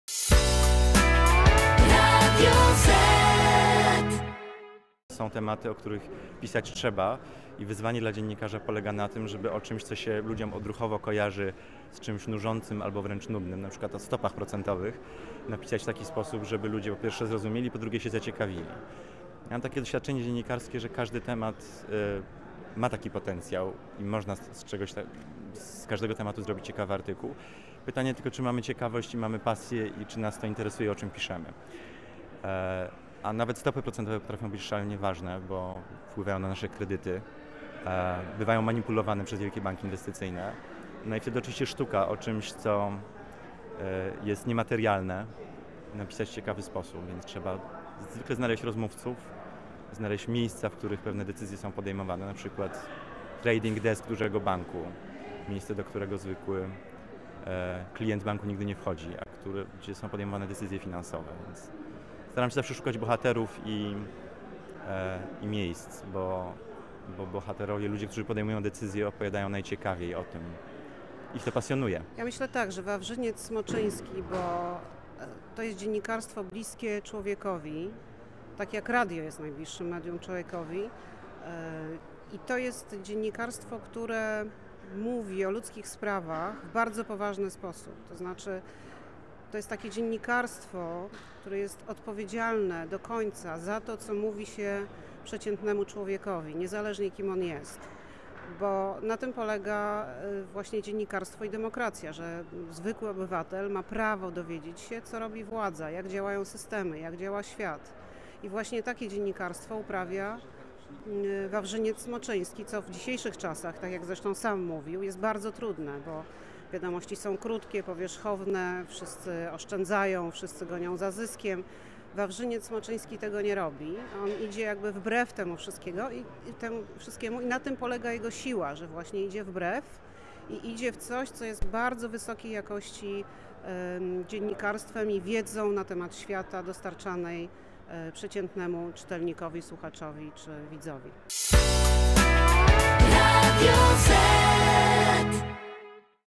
Wawrzyniec Smoczyński laureatem Nagrody im. Woyciechowskiego / Nagroda Radia ZET
Redaktor tygodnika "Polityka" został nagrodzony za cykl publikacji wnikliwie przedstawiających tematykę polityki i gospodarki międzynarodowej, objaśniających w sposób niezwykle kompetentny i dziennikarsko atrakcyjny przyczyny, przebieg i konsekwencje światowego kryzysu finansowego.